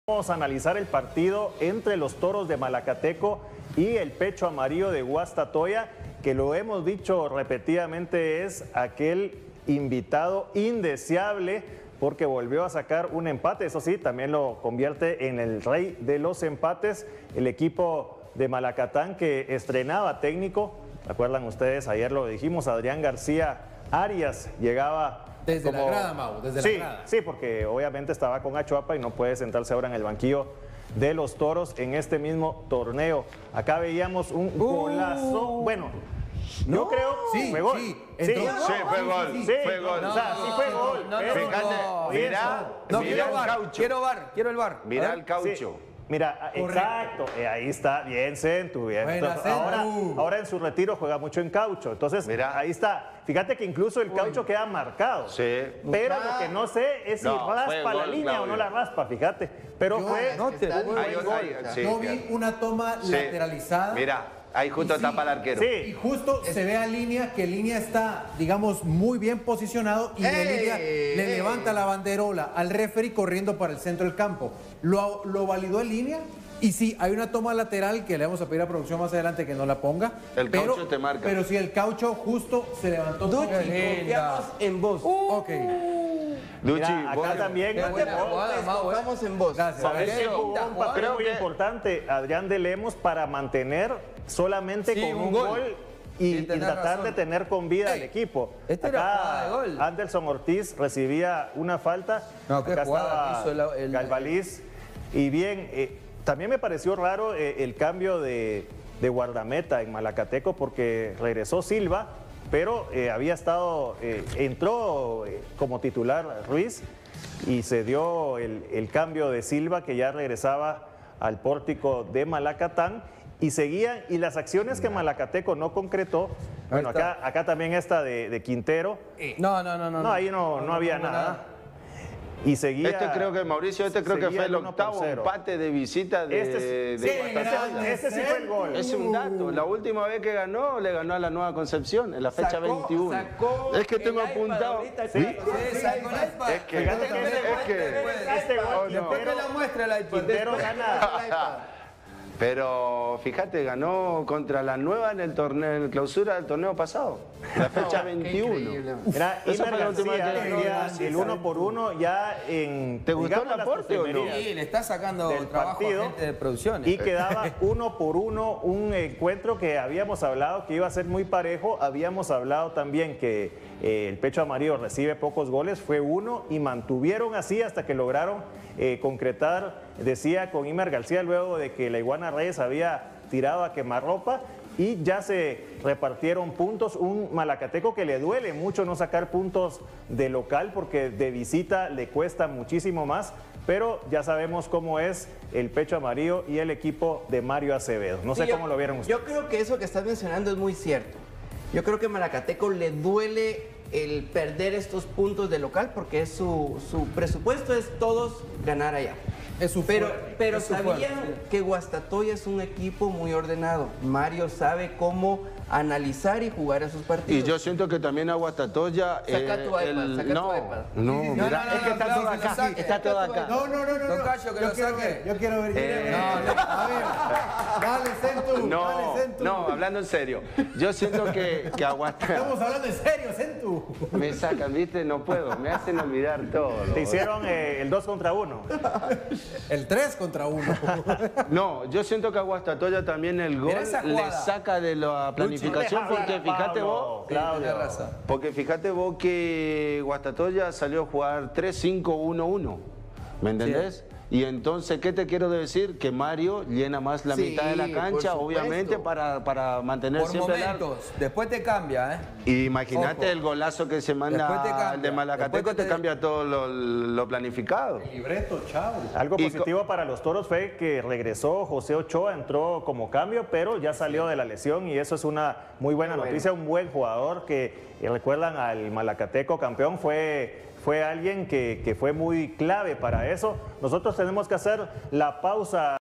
Malacateco y Guastatoya empataron a 1 en Malacatan
Toros y pecho amarillos repartieron puntos en su juego por la jornada 12.